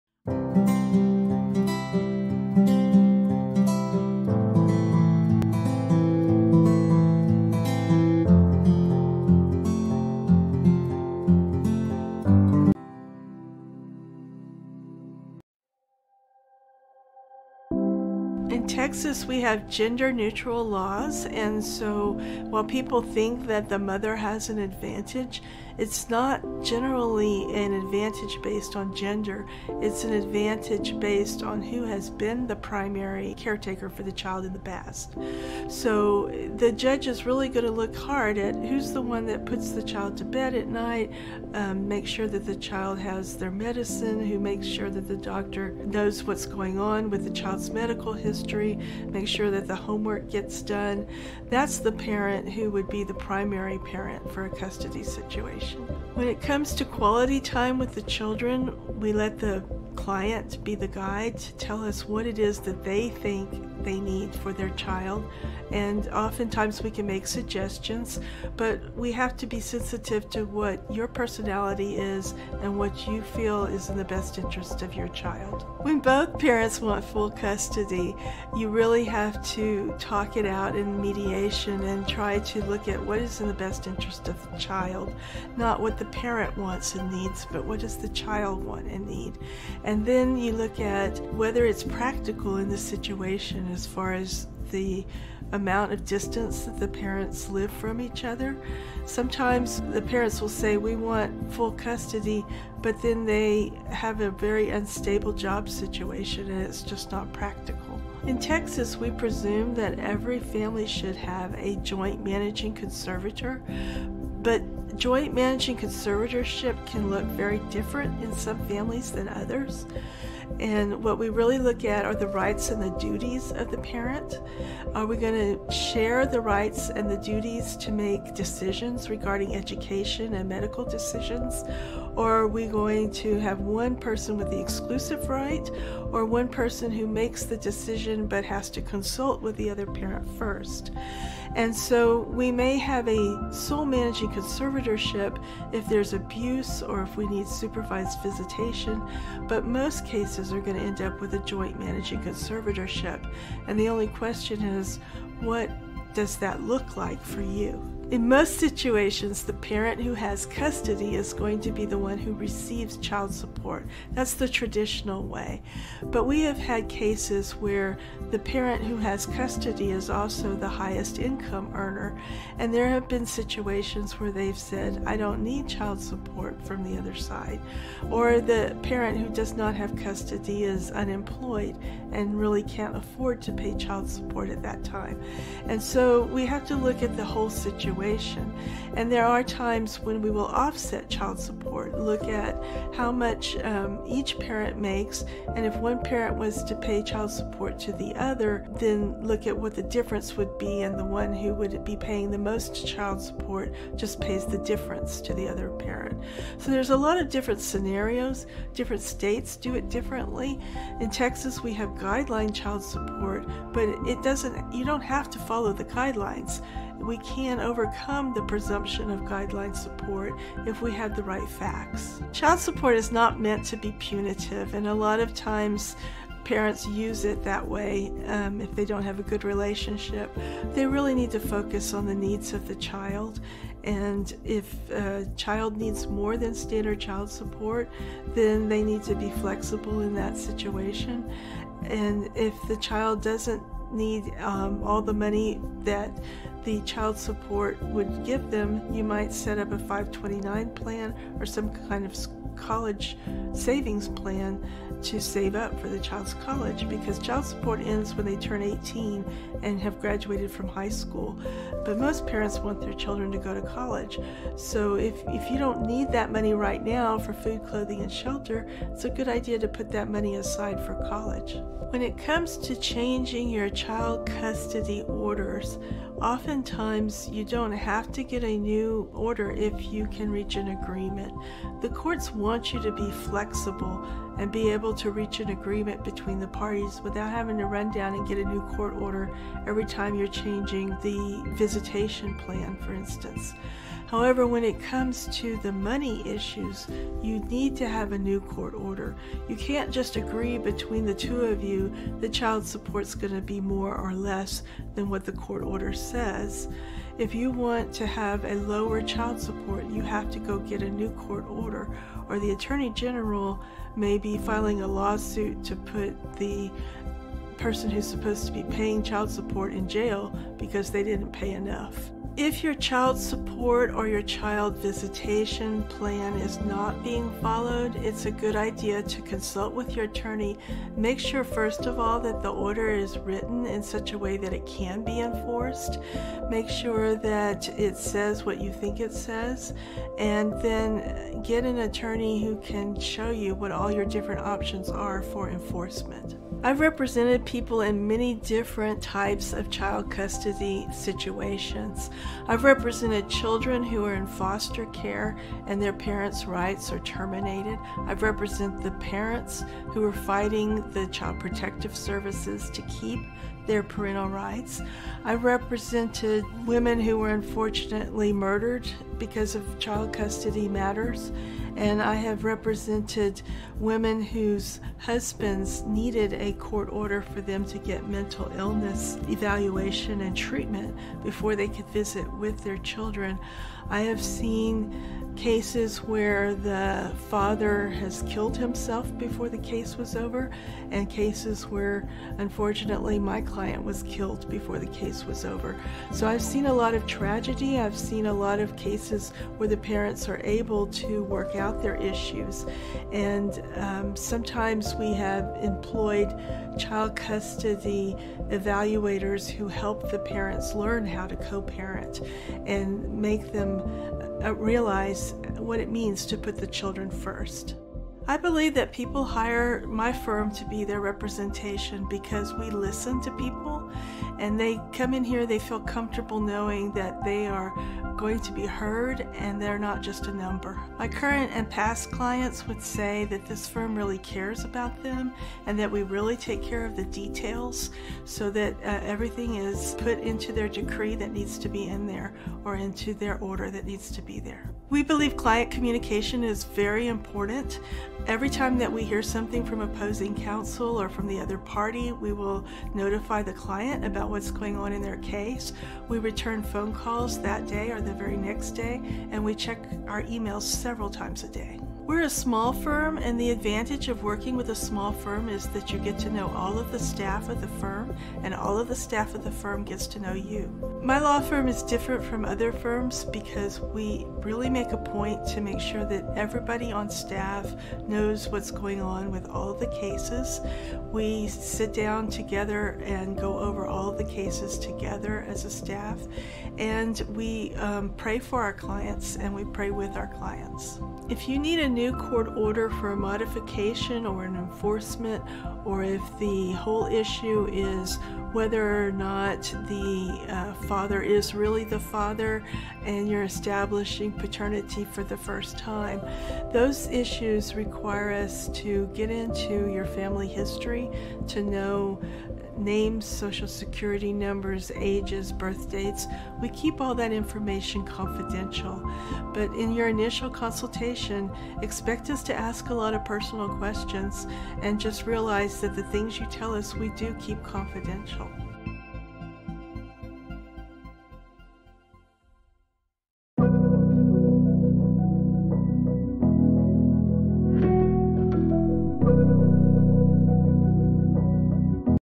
Conservatorship | Support & Modifications | Laura D. Heard
Under U.S. law, conservatorship is the appointment of a guardian or a protector by a judge to manage the financial affairs and/or daily life of another person due to old age or physical or mental limitations. A person under conservatorship is a "conservatee", a term that can refer to an adult. A person under guardianship is a "ward", a term that can also refer to a minor child. Conservatorship may also apply to corporations and organizations.

The conservator may be only of the "estate" (financial affairs), but may be also of the "person", wherein the conservator takes charge of overseeing the daily activities, such as health care or living arrangements of the conservatee. A conservator of the person is more typically called a legal guardian.

If you have questions or inquiries you can visit our